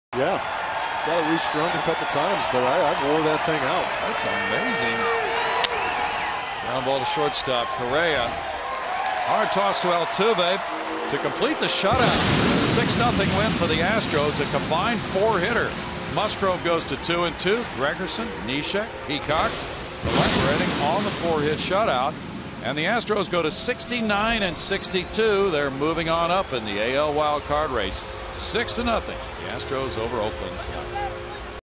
OAK@HOU: Peacock retires Butler as Astros win, 6-0
Brad Peacock gets Billy Butler to ground into a forceout, finishing the combined four-hit shutout as the Astros win, 6-0